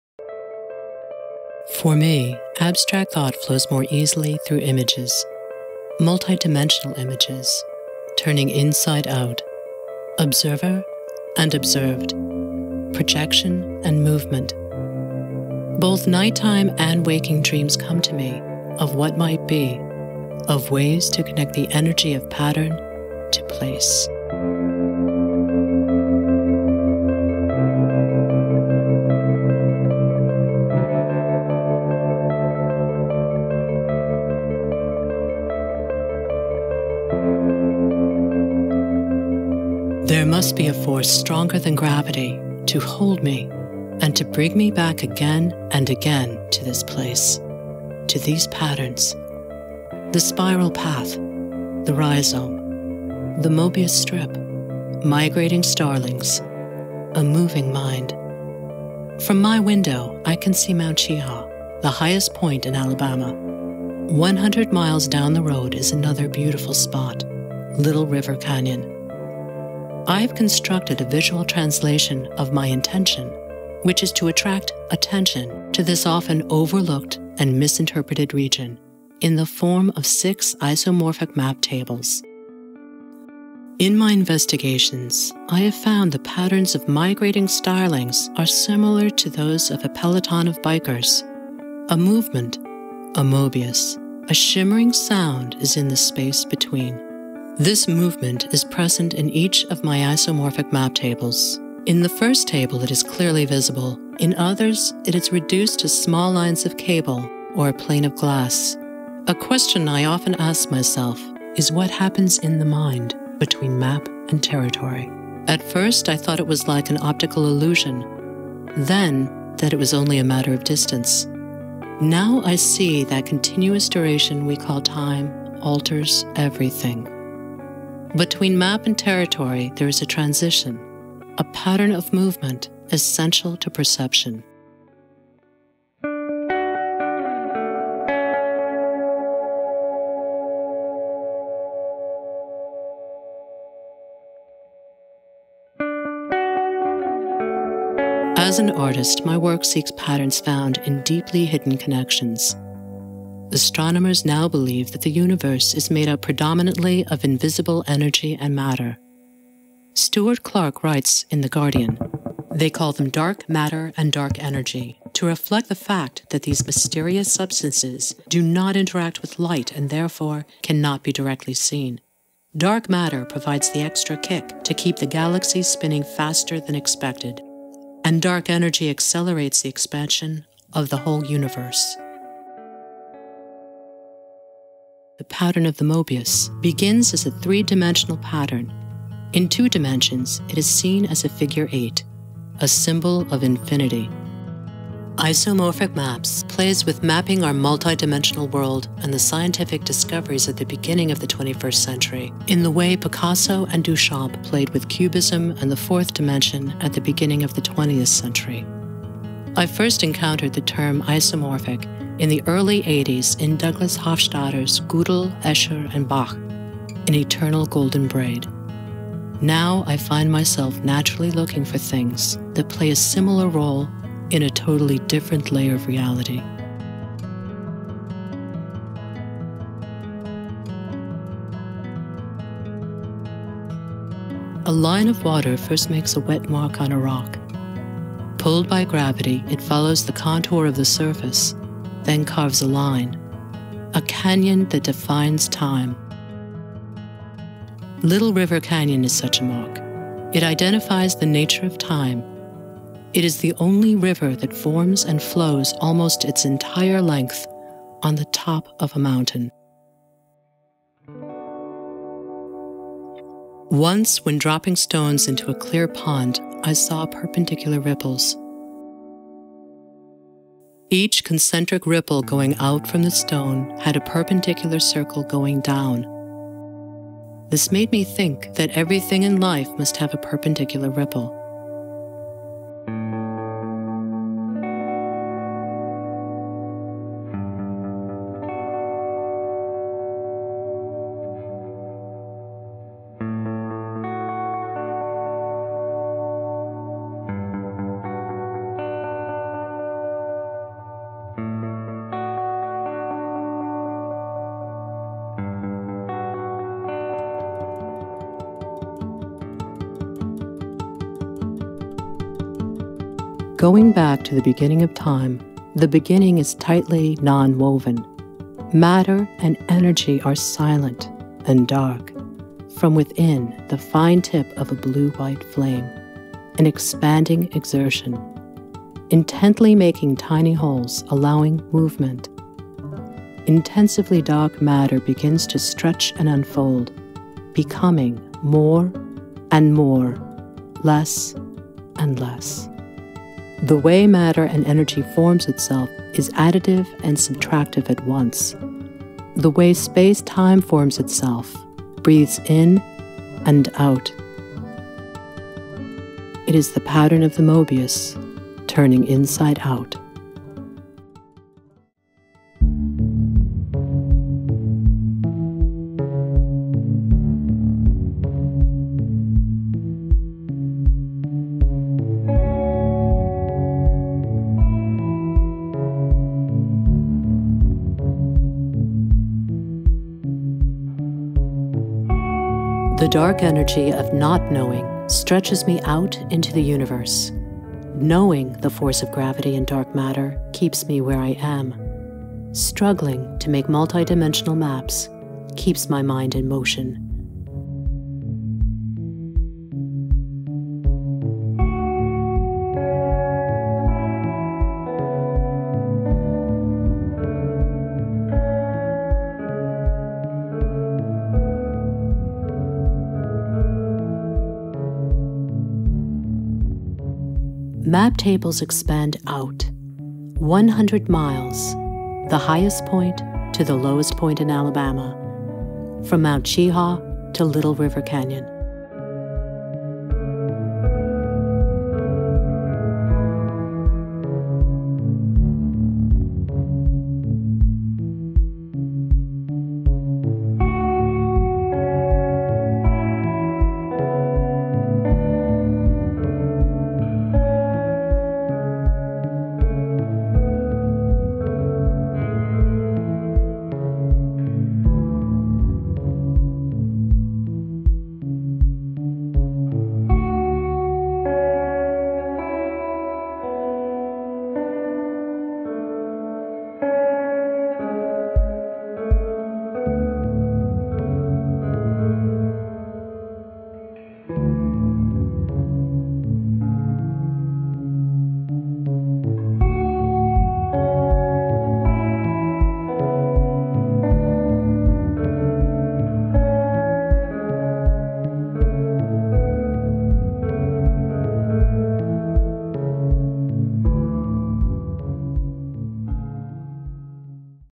Patricia Boinest Potter: Patterns of Place
Pat Potter creates enigmatic artworks in the form of three-dimensional maps that she refers to as Isomorphic Maps. Ostensibly representing a one-hundred-mile stretch of northern Alabama, these works also expand outward to the cosmos, then inward again into the dark energy of particle physics. The series, called Patterns of Place, offers a tantalizing mix of technical experimentation, metaphoric expansiveness, and curiosity in every square inch. Born in Charleston, but a longtime resident of Alabama, Potter has developed her own artmaking techniques, informed by her extensive background in architecture. Through the development of these idiosyncratic techniques, she has found ways to give unique expression to the ideas that animate her.